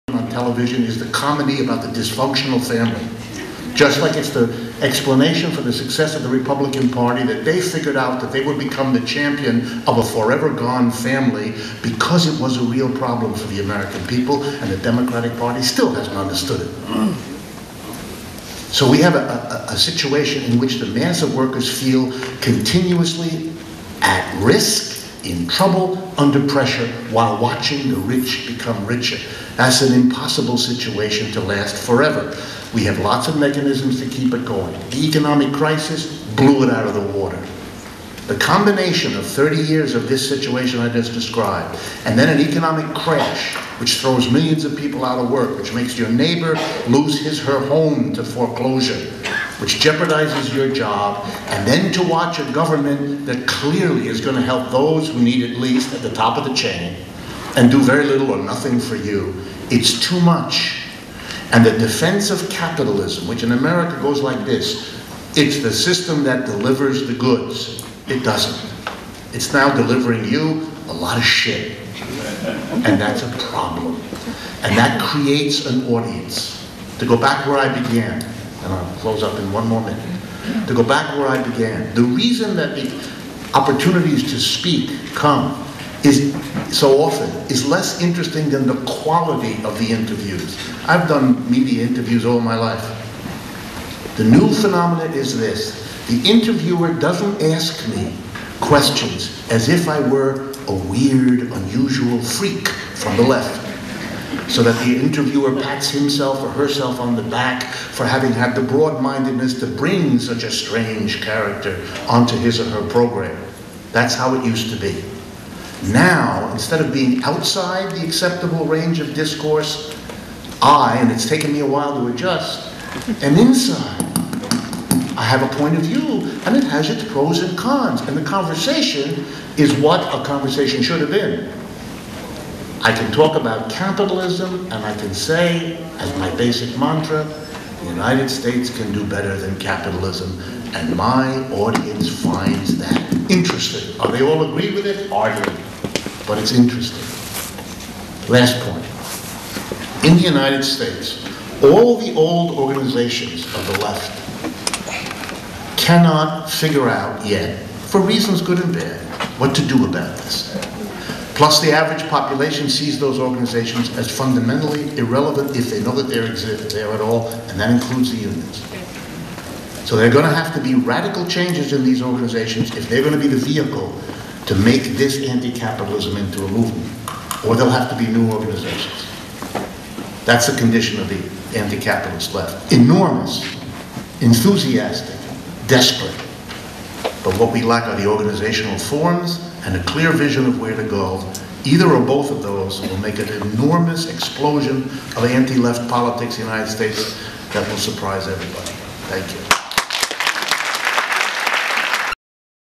Left Forum: The Anticapitalist Left in the World Today (part 2)
Marcello Musto- Chair
Baris Karaagac
Johanna Brenner
Luciana Castellina
Michael Kratke
Mochael Lowy
Richard D. Wolff
Seongjin Jeong
Shuangli Zhang
 and Teivo Teivainen discuss motivation, and justification for anticapitalist sentiments.